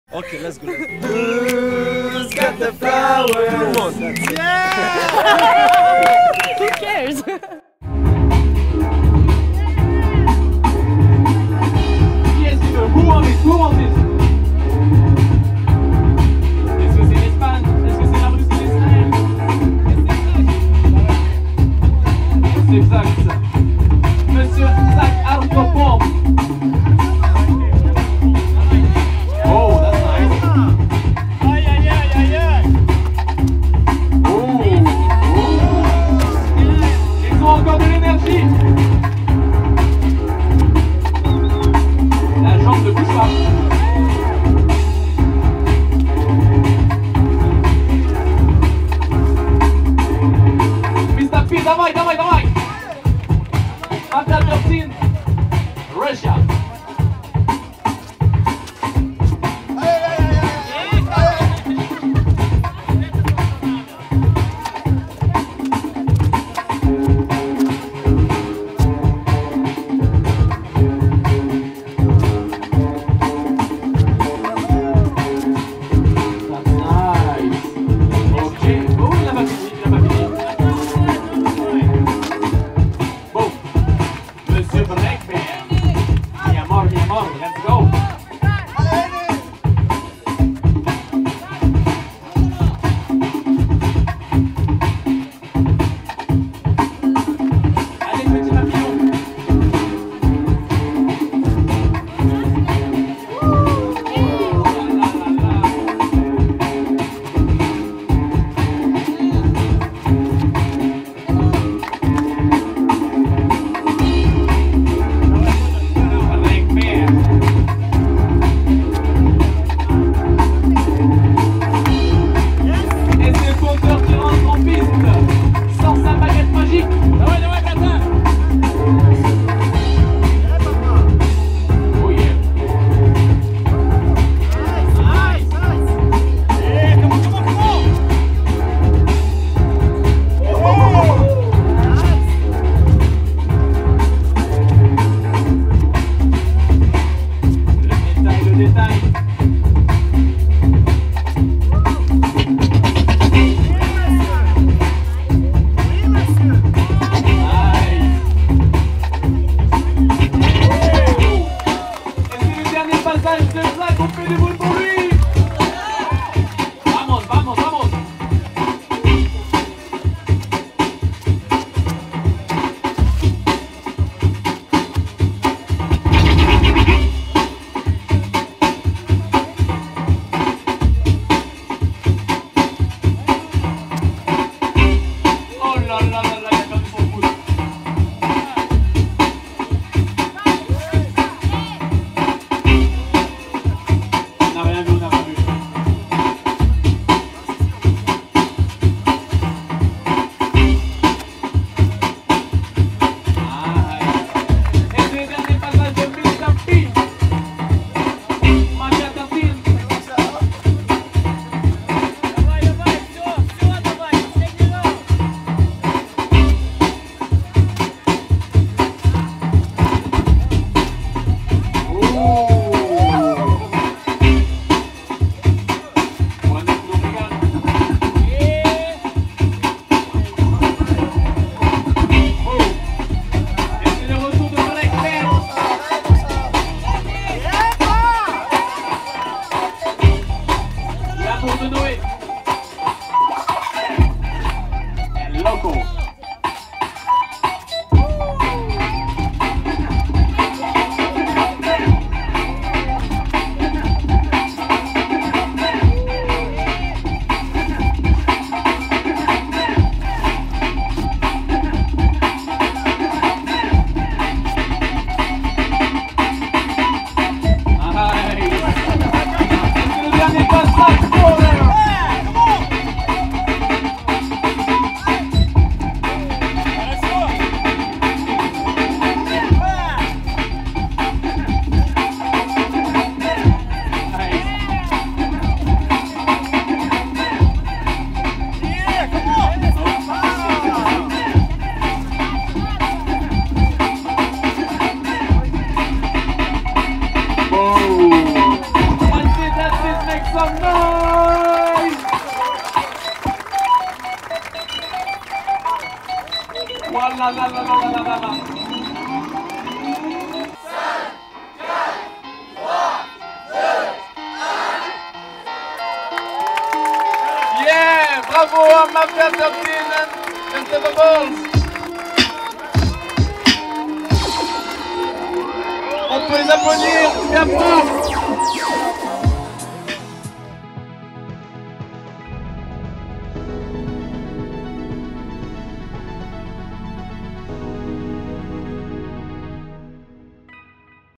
BULLZ MAFIA 🥝 XAK & BERN • Finale Footwork 2018
2vs2 Footwork Finals at Who got the flower ?! 2018
Bullz Mafia vs Xak & Bern

Judges:
• Kate (South Front)
• Noé (Nextape)
• Maher (Tekken

The Colab • 3vs3 Battle • Junior 1vs1 • 2vs2 Footwork • 1vs1 Toprock